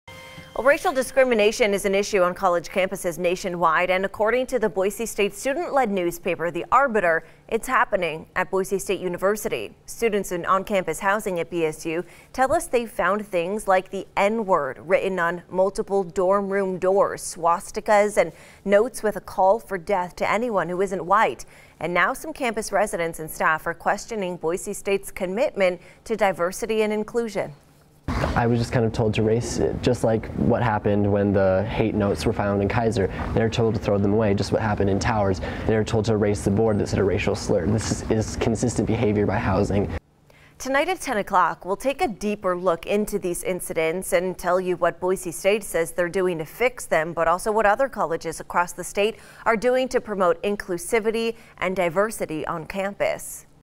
TEASE: Boise State students are calling on administration to address incidents of racism on campus
After several incidents of on-campus racism at Boise State University, students are calling on administration to do more to put an end to incidents of racism on campus.